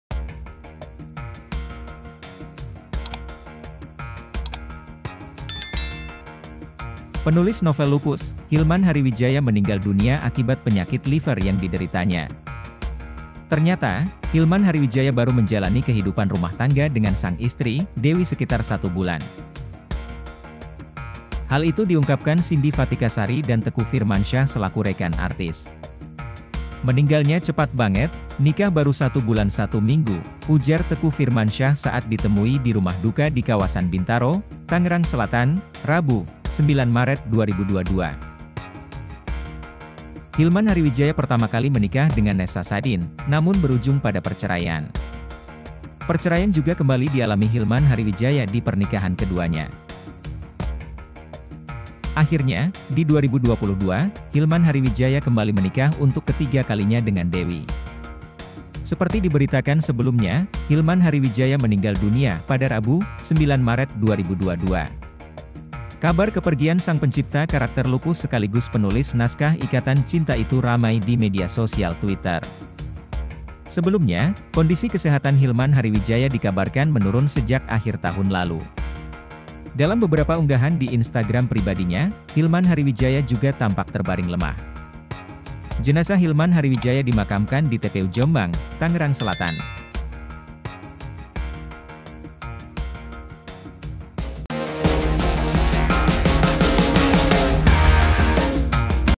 Padahal Baru Menikah Sebulan, Hilman Hariwijaya Meninggal Dunia
Penulis novel Lupus, Hilman Hariwijaya meninggal dunia akibat penyakit liver yang dideritanya.
Ternyata, Hilman Hariwijaya baru menjalani kehidupan rumah tangga dengan sang istri, Dewi sekitar satu bulan.

Hal itu diungkapkan Cindy Fatikasari dan Teuku Firmansyah selaku rekan artis.
"Meninggalnya cepat banget, nikah baru satu bulan satu minggu," ujar Teuku Firmansyah saat ditemui di rumah duka di kawasan Bintaro, Tangerang Selatan, Rabu (9/3/2022).